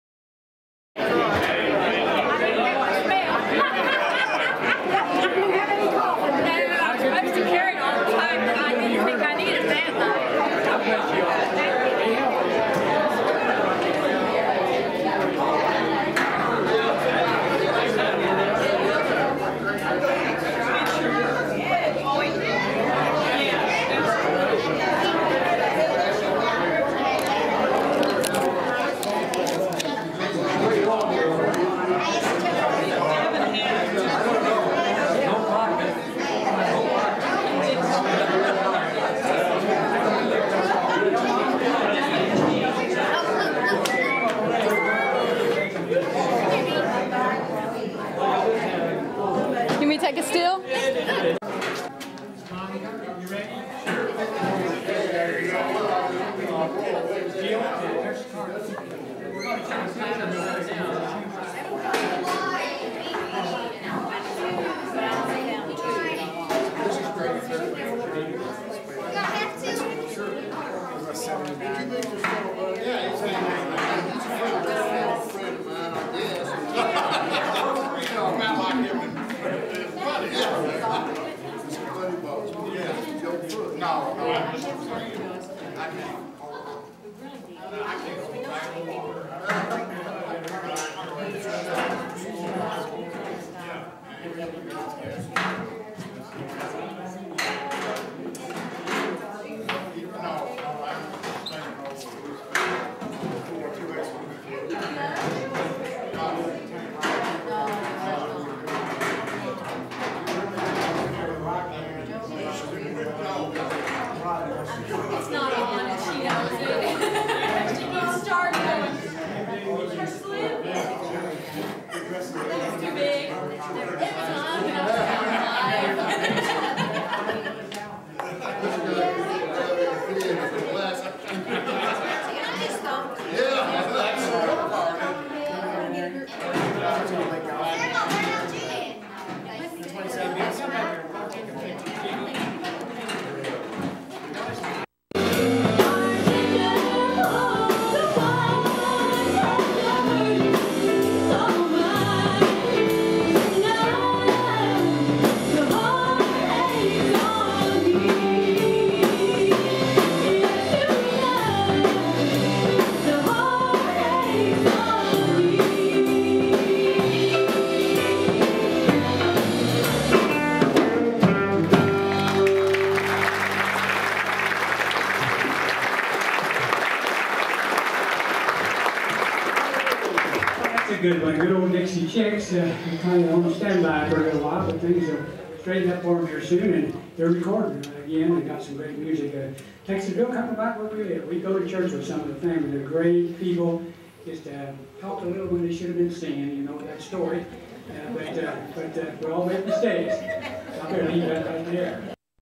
Jill Carla Kali Ben Union Mill Opry road show
after the show plus Jill singing. Hallie Gini Buddie kali Ben Jill Carla